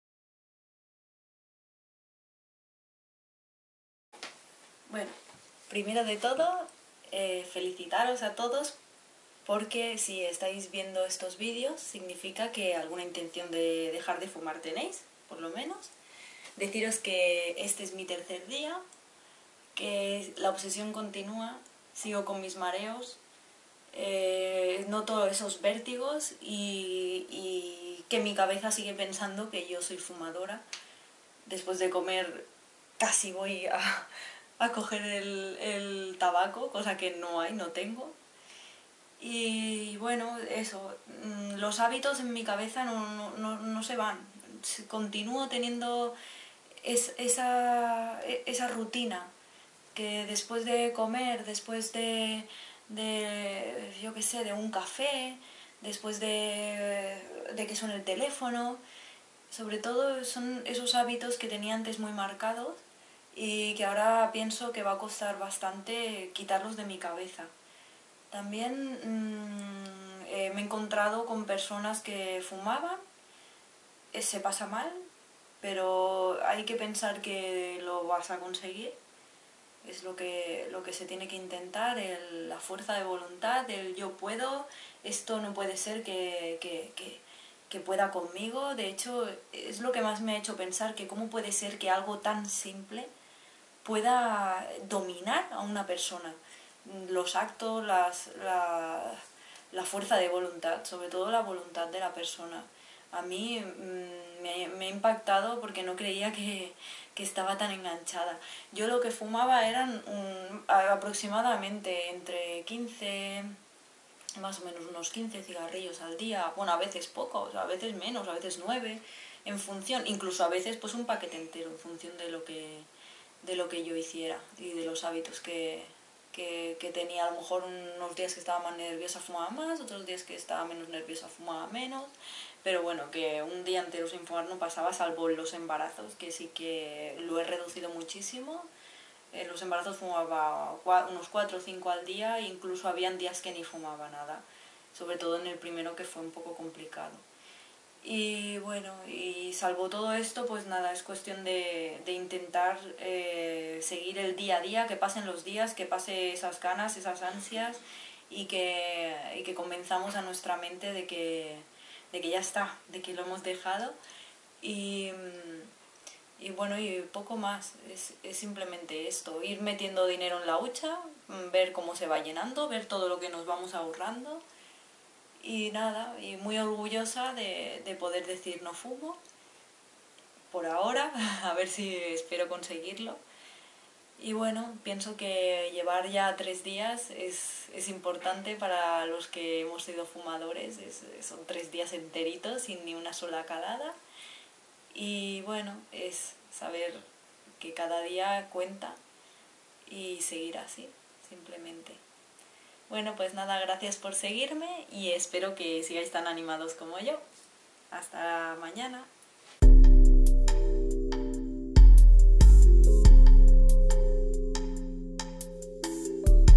Dejar de fumar día 3 // Quit 3rd
The third day is more of the same ... The obsession continues but we have to overcome, adding days, filling the piggy bank, encouragement to everyone!

El tercer día es más de lo mismo... La obsesión continúa pero tenemos que vencerla, sumando días, llenando la hucha, ánimo a todos!!